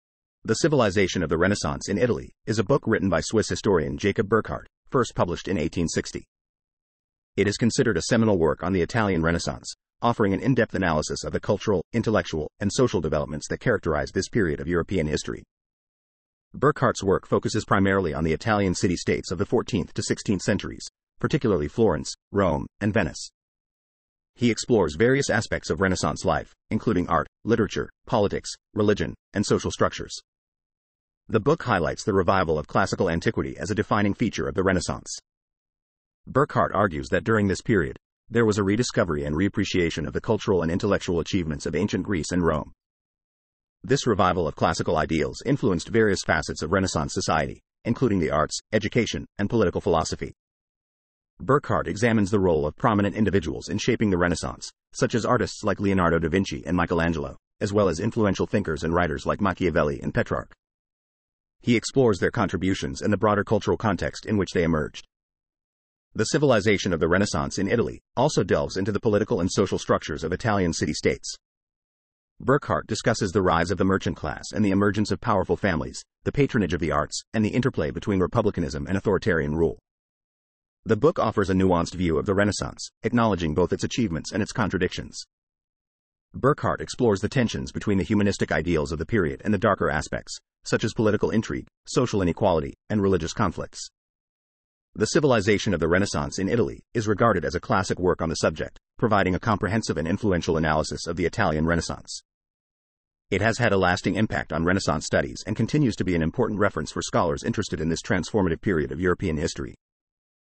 The Civilization of the Renaissance in Italy by Jacob Burckhardt | Summary
"""The Civilization of the Renaissance in Italy"" is a book written by Swiss historian Jacob Burckhardt, first published in 1860. It is considered a seminal work on the Italian Renaissance, offering an in-depth analysis of the cultural, intellectual, and social developments that characterized this period of European history.

Burckhardt's work focuses primarily on the Italian city-states of the 14th to 16th centuries, particularly Florence, Rome, and Venice. He explores various aspects of Renaissance life, including art, literature, politics, religion, and social structures.

The book highlights the revival of classical antiquity as a defining feature of the Renaissance. Burckhardt argues that during this period, there was a rediscovery and reappreciation of the cultural and intellectual achievements of ancient Greece and Rome. This revival of classical ideals influenced various facets of Renaissance society, including the arts, education, and political philosophy.

Burckhardt examines the role of prominent individuals in shaping the Renaissance, such as artists like Leonardo da Vinci and Michelangelo, as well as influential thinkers and writers like Machiavelli and Petrarch. He explores their contributions and the broader cultural context in which they emerged.

""The Civilization of the Renaissance in Italy"" also delves into the political and social structures of Italian city-states. Burckhardt discusses the rise of the merchant class and the emergence of powerful families, the patronage of the arts, and the interplay between republicanism and authoritarian rule.

The book offers a nuanced view of the Renaissance, acknowledging both its achievements and its contradictions. Burckhardt explores the tensions between the humanistic ideals of the period and the darker aspects, such as political intrigue, social inequality, and religious conflicts.

""The Civilization of the Renaissance in Italy"" is regarded as a classic work on the subject, providing a comprehensive and influential analysis of the Italian Renaissance. It has had a lasting impact on Renaissance studies and continues to be an important reference for scholars interested in this transformative period of European history."